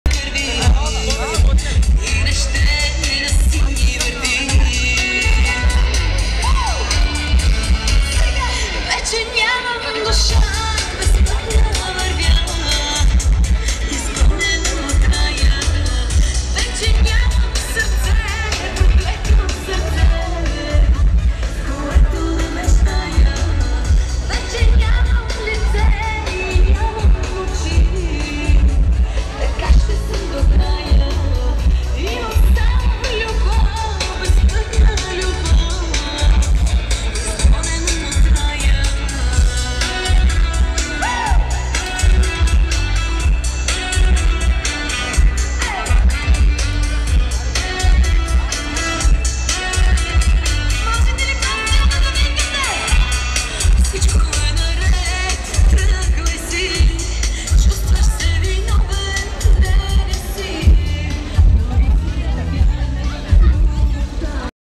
Културен шок и поп-фолк ужас в сърцето на София!
На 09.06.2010 в сърцето на София със съдействието на столична община и с разрешението на кмета Фандъкова се състоя поругаване на българската културна идентичност. ТРЯБВА ЛИ ДА ОСТАНЕМ БЕЗУЧАСТНИ???